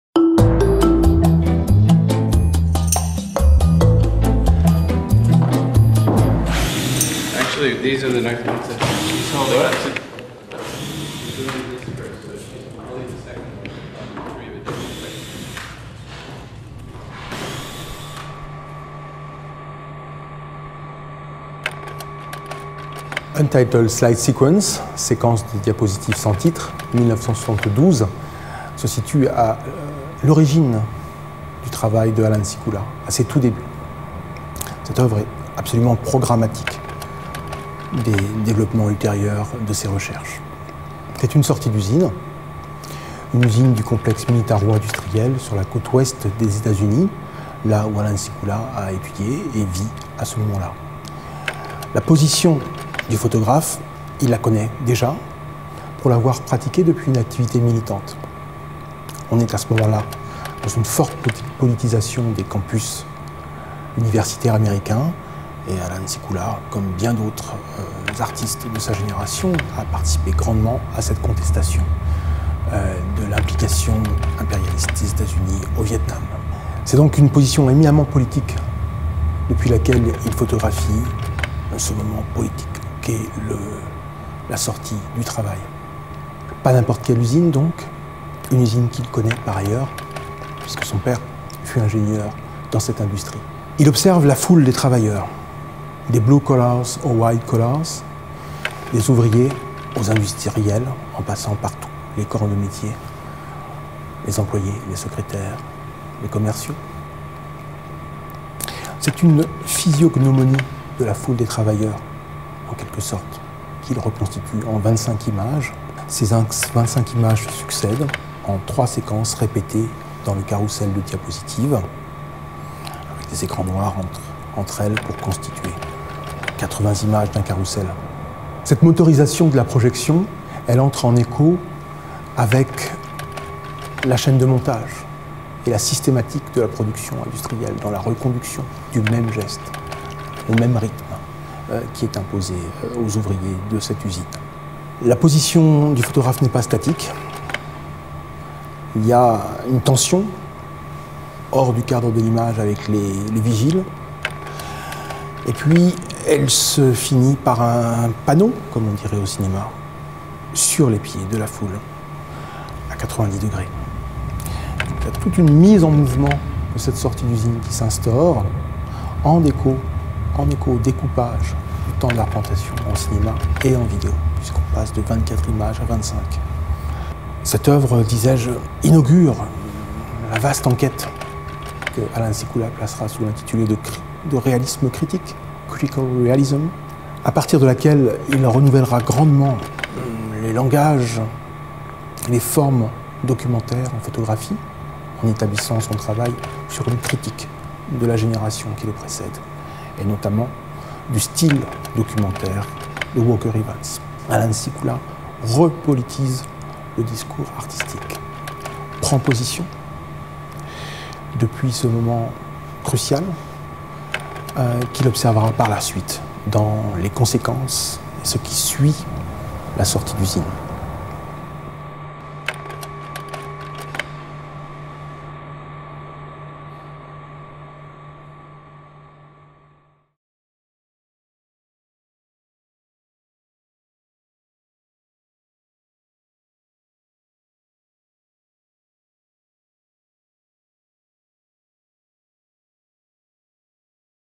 Allan Sekula, Untitled Slide Sequence - par Pascal Beausse (VOSTeng)
Allan Sekula observe une foule de travailleurs à la sortie d’une usine. 25 images se succèdent en 3 séquences répétées dans le carrousel de diapositives. Cette motorisation de la projection rappelle notamment une chaîne de montage.

Réalisation : 2016